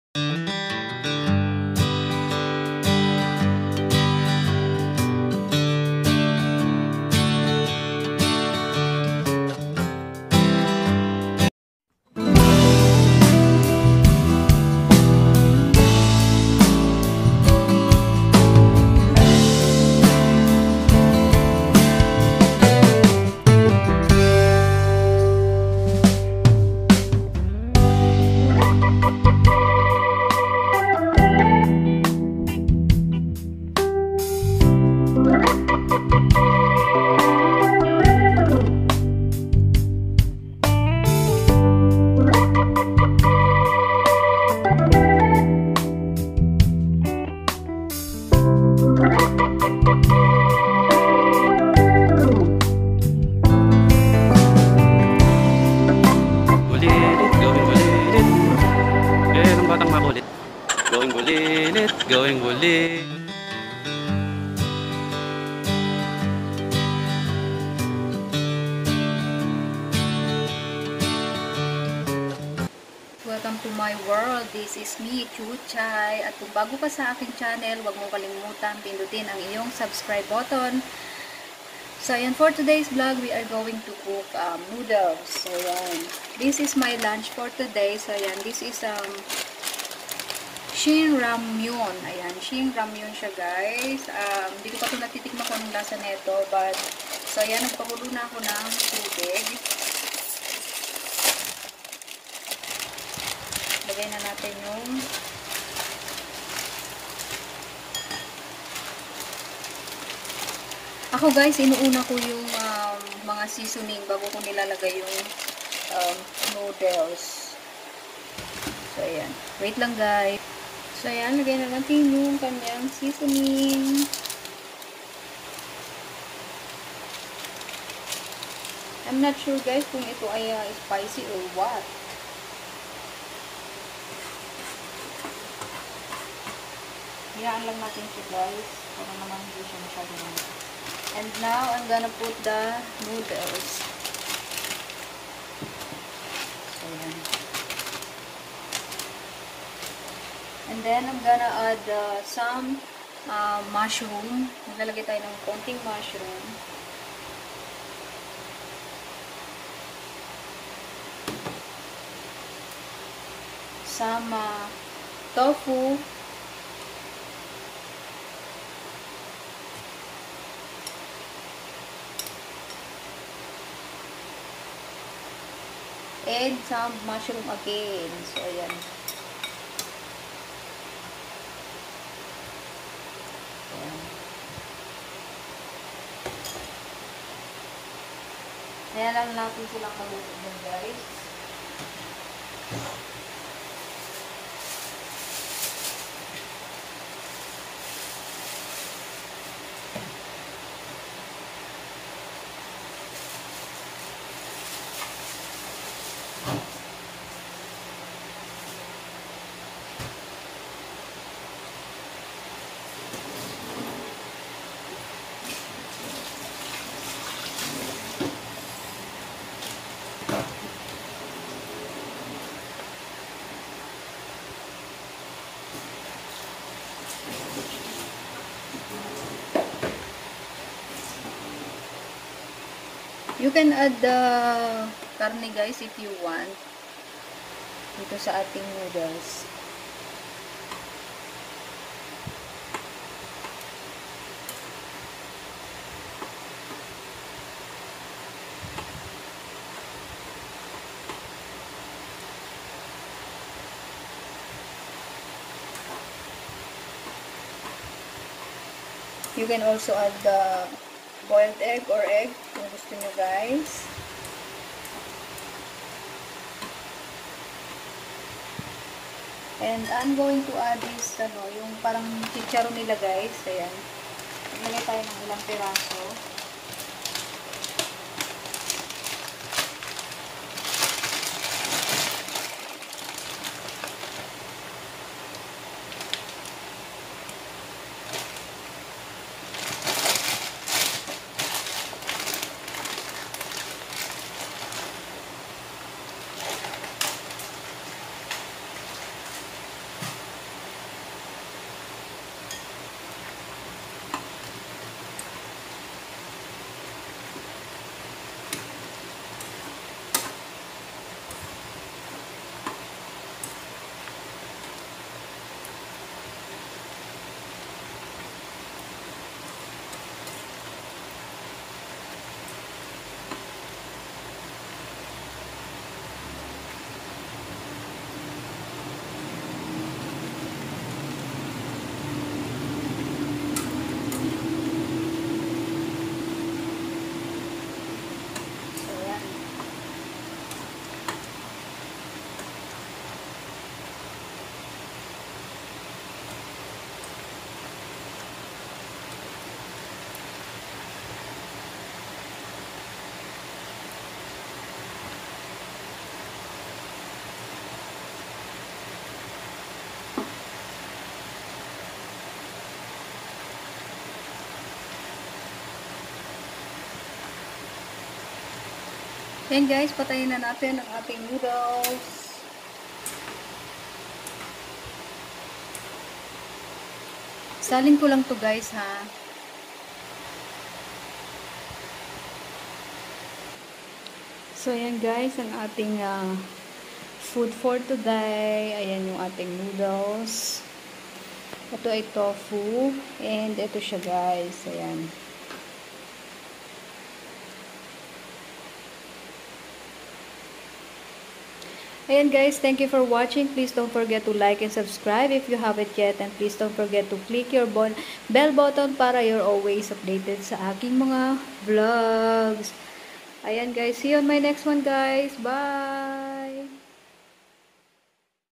SHIN RAMYUM RAMEN COOKBANG |chutchai world vlog
Hi everyone another COOKING vlog for today,  let's try this Taiwanese Shin Ramyum Ramen/Noodles for the first time. But with a twist to give more flavor to it.